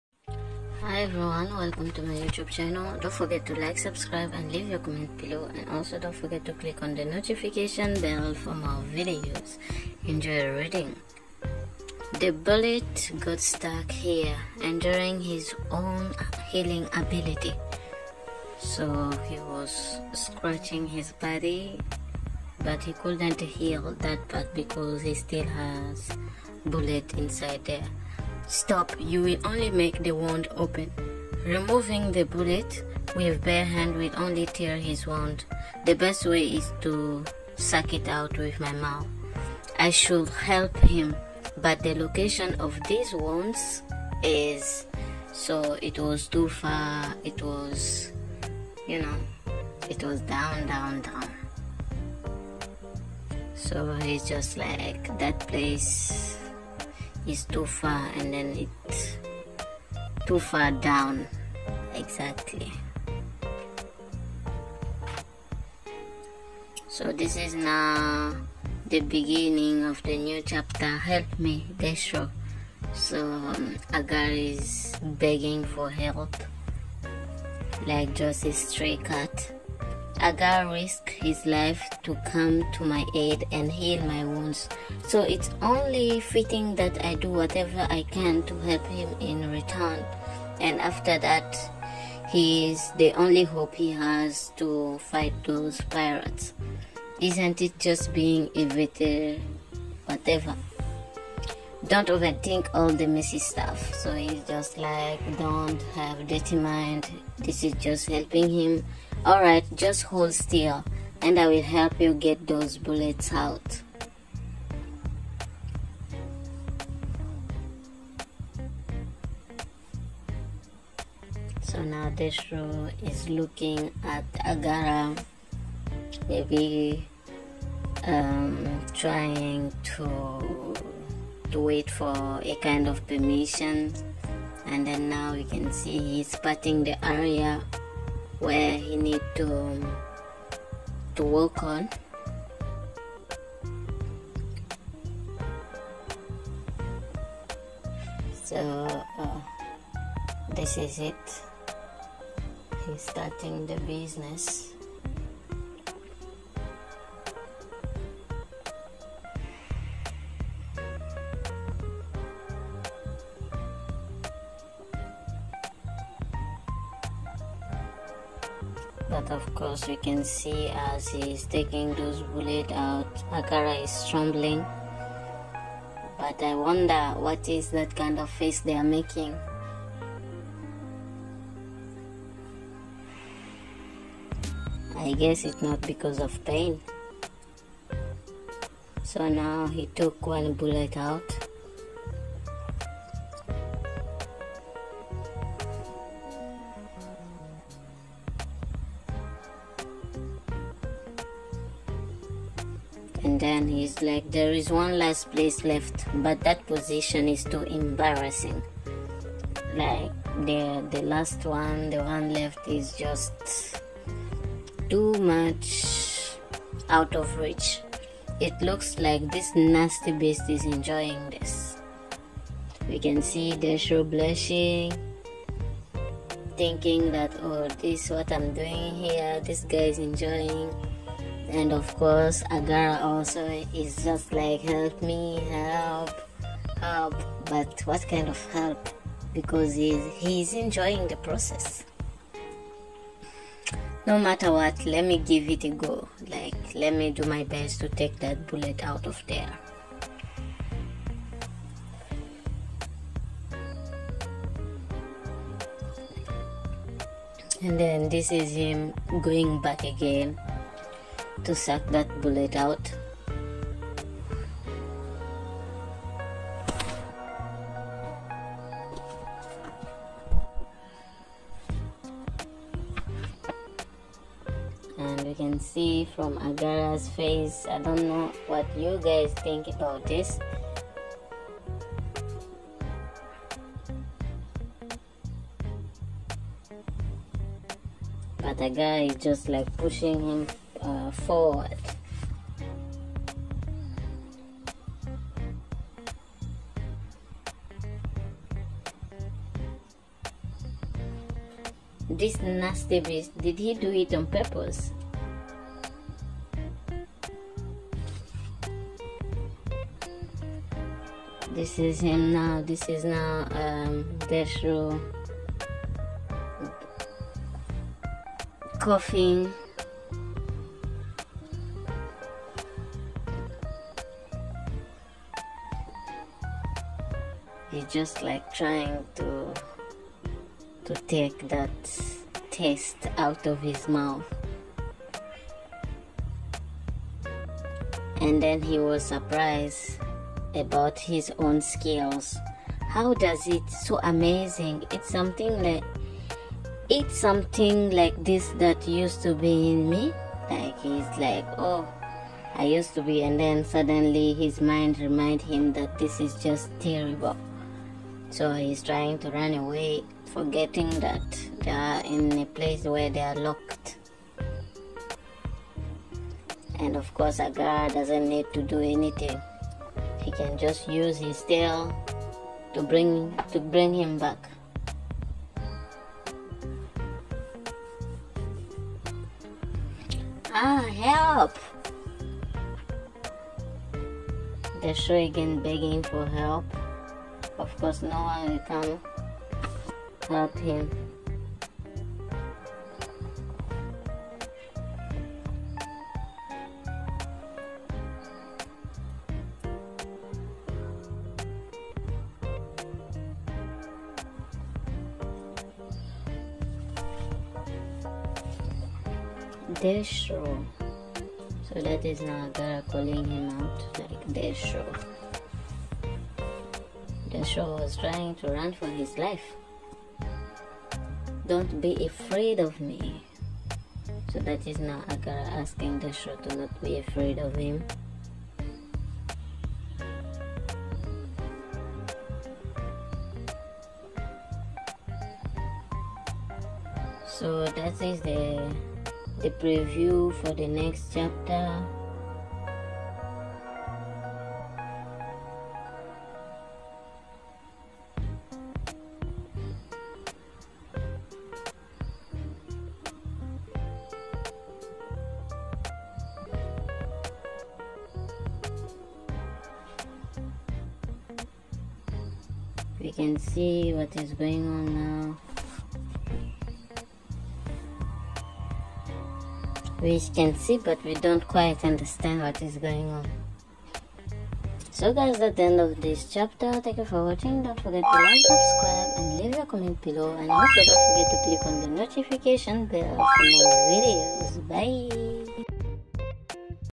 [BL] The Merman Spouse Chapter 27
Desharow Merman | The Merman Husband

Desharow, a fourth-year biology major at the St. Petersburg Naval Academy, and his mentor Rhein went on a research mission when they came upon a curious black-scaled mermaid in the vicinity of Iceland. Desharow, who was engrossed in his academic work and completely unaware that he had already become the mermaid's prey, was immediately mesmerized by the strong and gorgeous creature.

Credits:
Artist: LiXiaoLi
Translator : AshScan
Voice Over : AshScan
Editor : AdhScan
Video by : AshScan